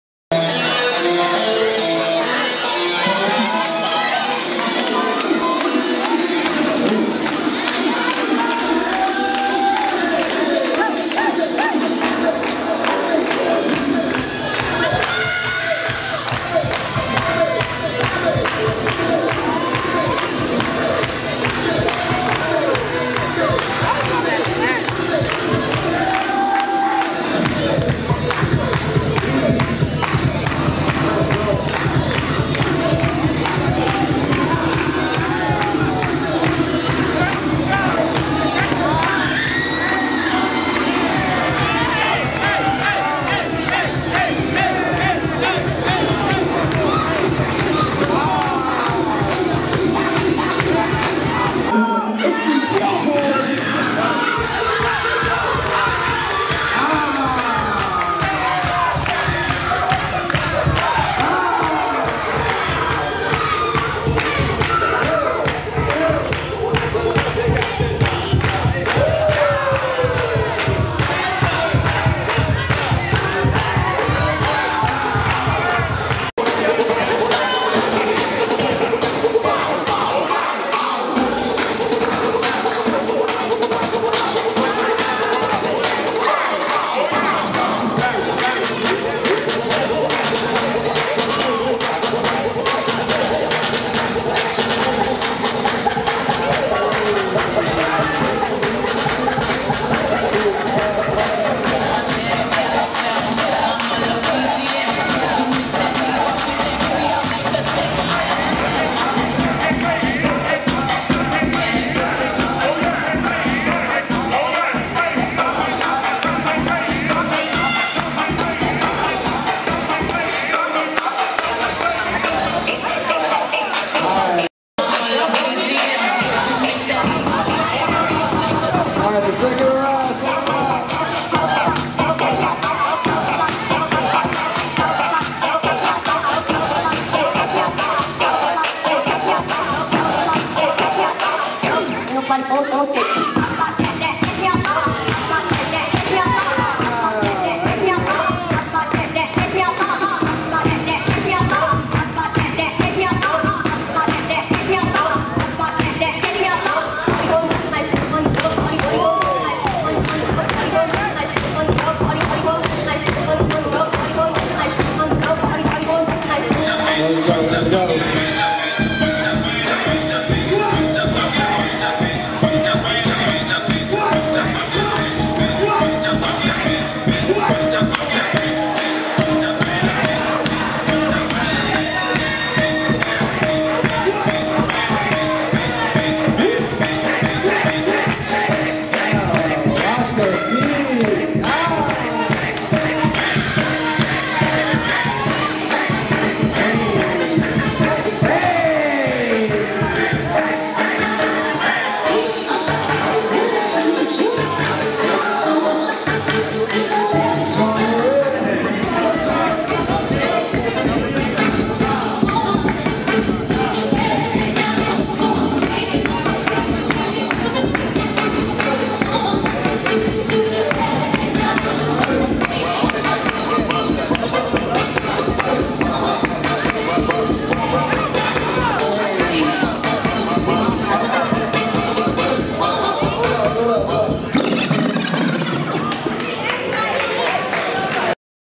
BmoreThan DanceThe League Event Kasheena Blasian ree v Runwayslayerzzz
BaltimoreClub Meets AllStyles A dance battle league centered around all styles of dance networking and battling in peace!
The Goal is for all dancers to connect and respect each others style! We will have